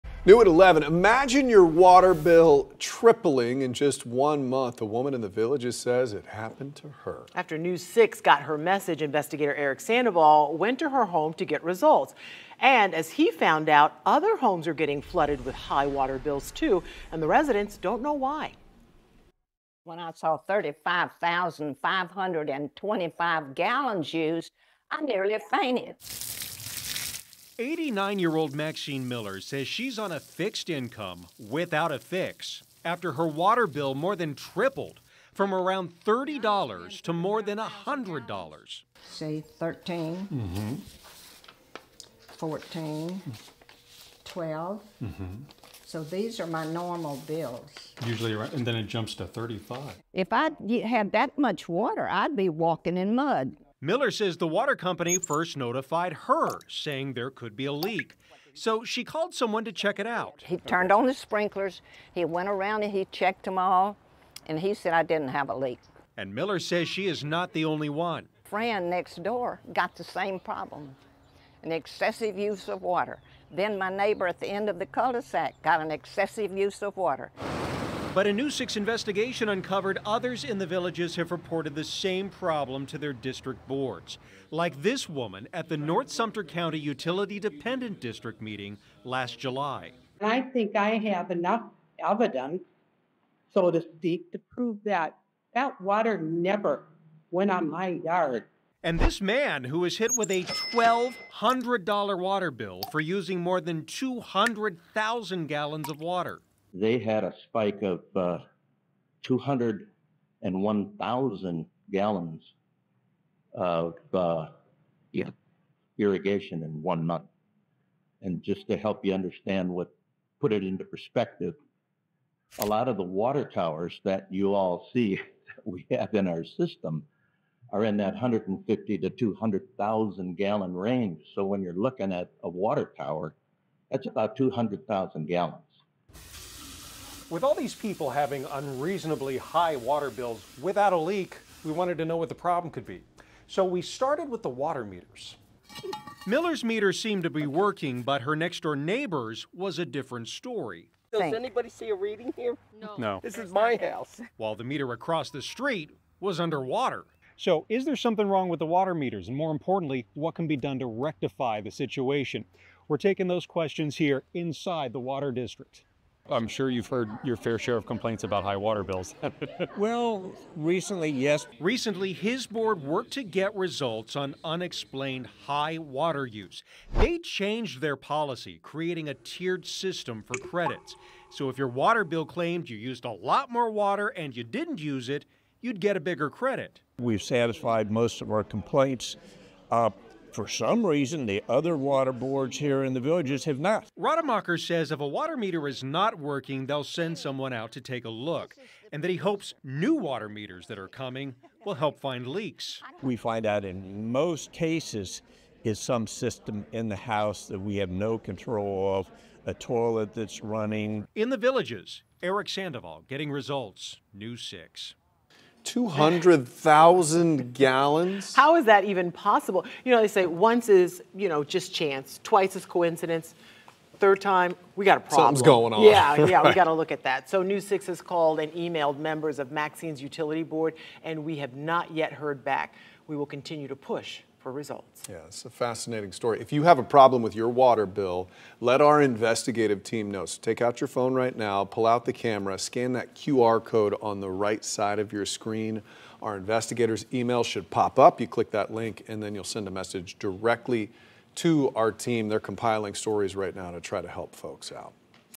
High water bills prompt News 6 investigation in The Villages
Maxine Miller says she is on a fixed income without a fix after her water bill more than tripled from around $30 to more than $100.